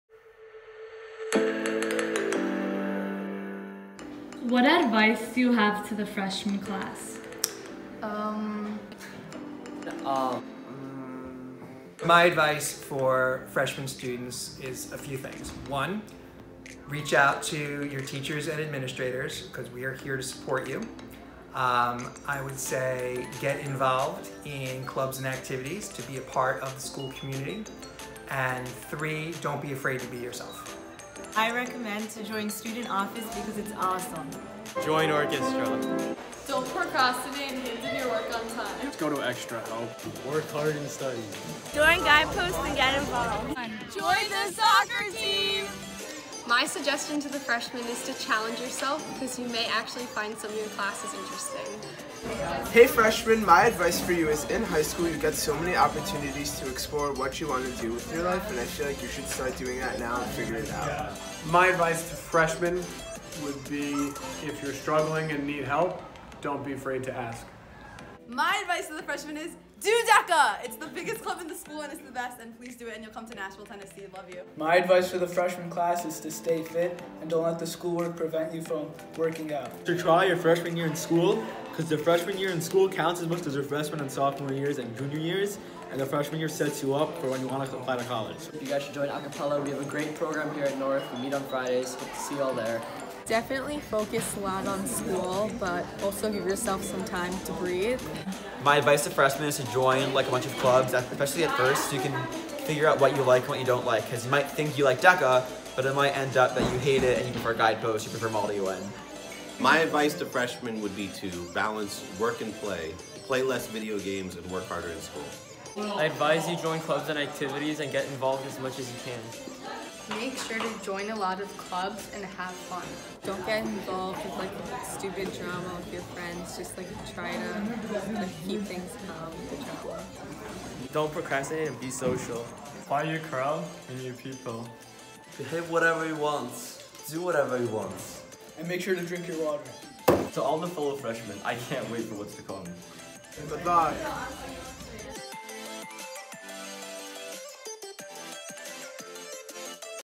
Advice to the Freshman Class | Great Neck North High School
As a Freshman, entering high school can be exciting but also intimidating. This video includes useful insights that Great Neck North students have shared for the Freshman class. Have a great year ahead!

Visit gnnguidepost.org for more intriguing content on what’s going on around the school.

Video by Nora Monasheri